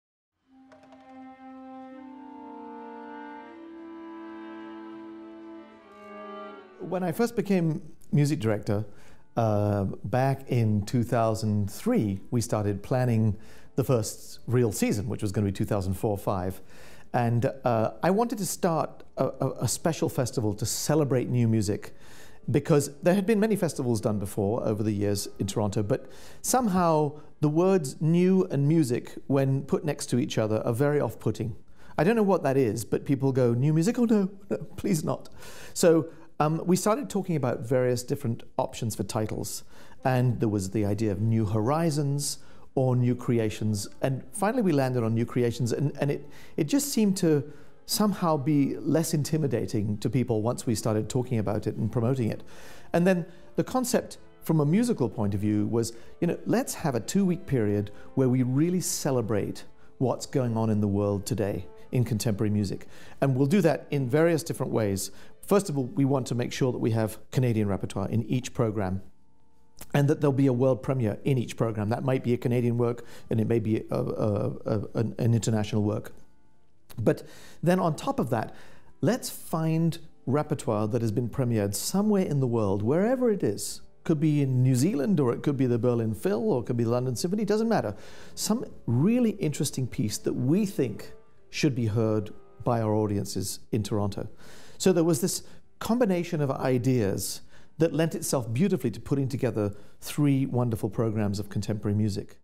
Music Director Peter Oundjian on The New Creations Festival
Music Director Peter Oundjian discusses the origins of the New Creations Festival. This year marks the 10th Anniversary of the festival.